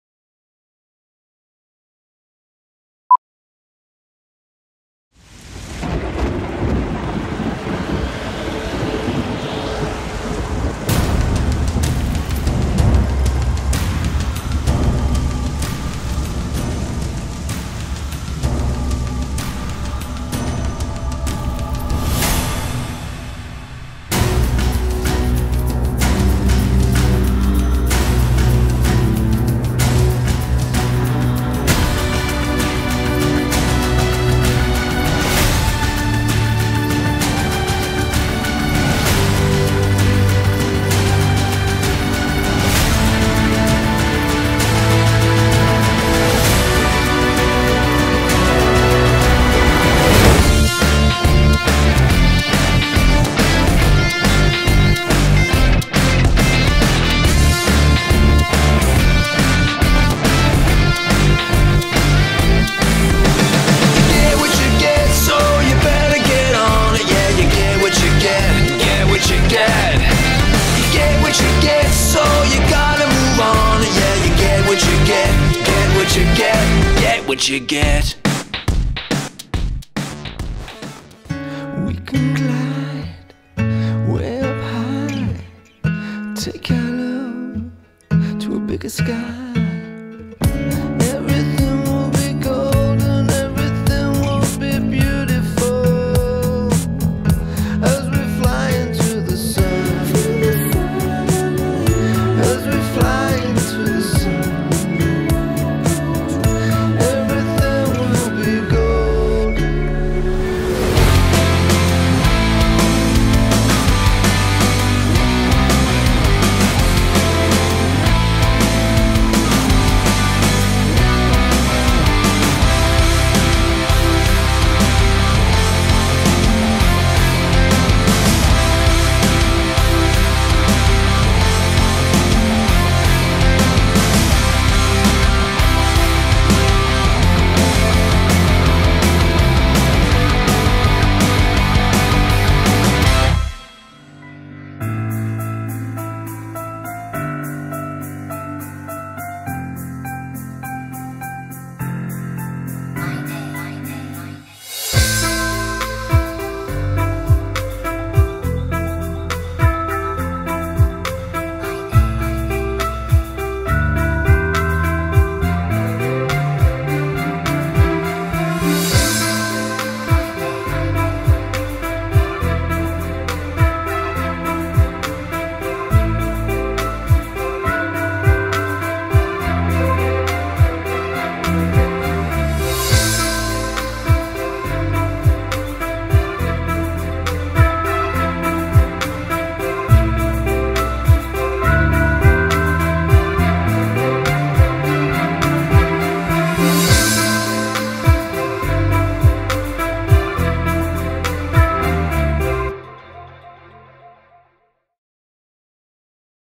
Trussardi MY NAME, dietro le quinte dello spot
Per il lancio di MY NAME, la maison Trussardi sceglie Gabriele Salvatores per raccontare una Milano nascosta e ricca di fascino.